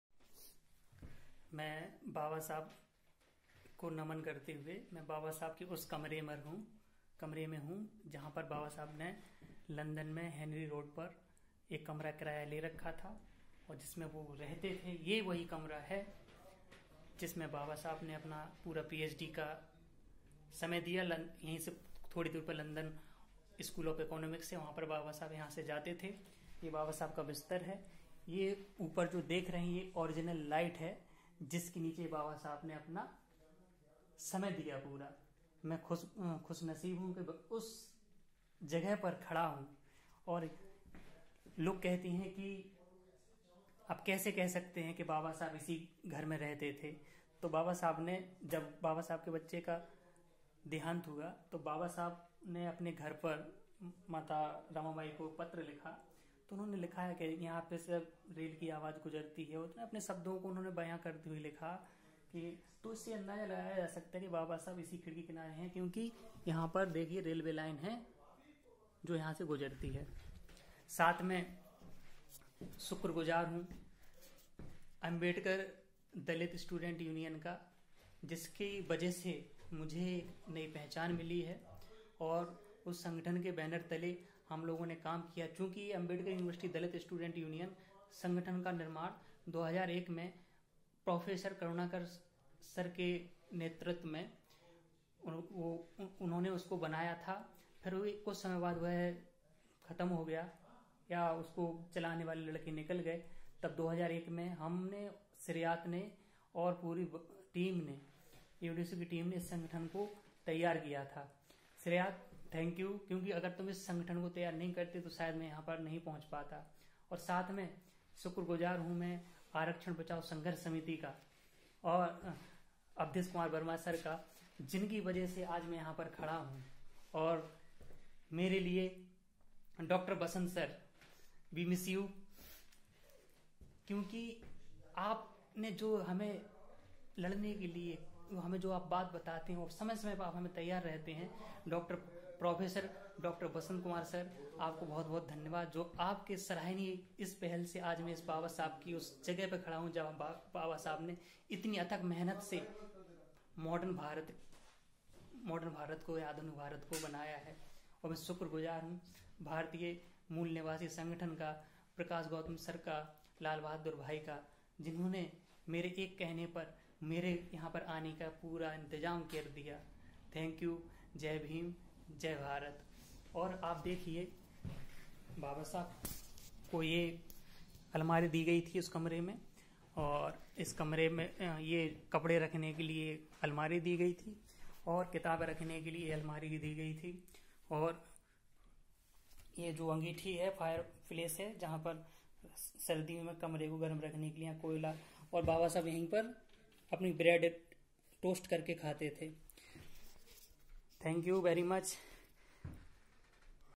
लंदन स्थित बाबासाहेब का घर जहां वह किराए से रहते थे वहां से सीधे अनछुए पहलुओं पर प्रकाश
Babasaheb Dr. Bhimrao Ambedkar ने लंदन स्कूल ऑफ इकोनॉमिक्स से पीएचडी की डिग्री ली थी। वहीं बाबासाहेब ने 10 किंग हेनरी रोड पर किराए से कमरा लिया था। उसी कमरे का वृतांत इस वीडियो में मैं डॉ अजय कुमार पीएचडी (रसायन शास्त्र)